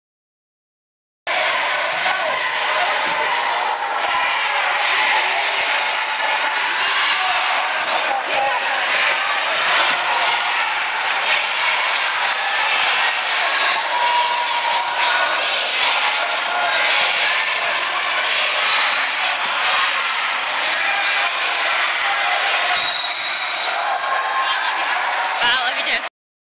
Basketball video 12
Bunch of random parts of the girls game against Sulphur. We won. 56-44. Btw, we are in white.